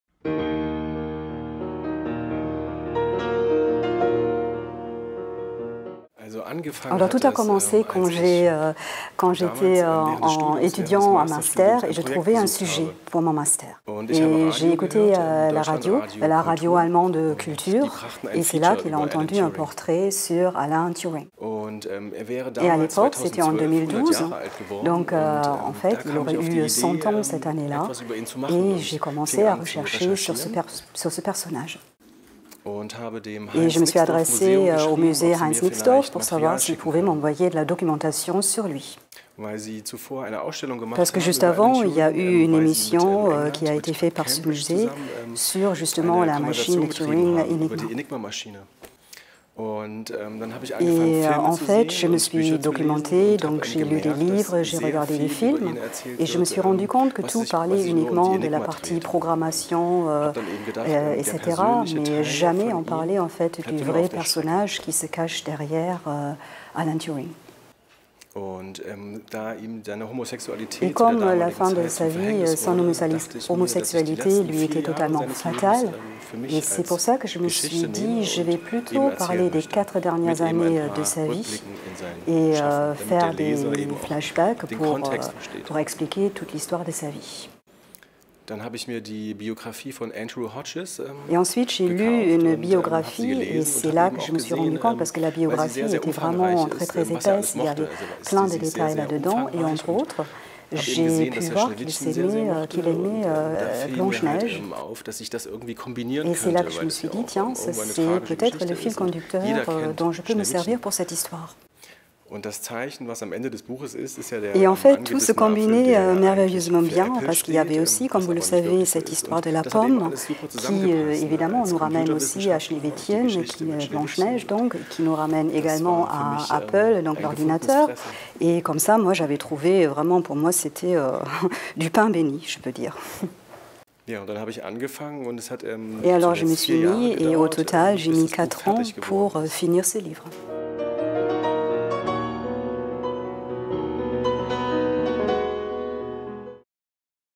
Robert Deutsch - Turing
Robert Deutsch vous présente son ouvrage "Turing" aux éditions Sarbacane.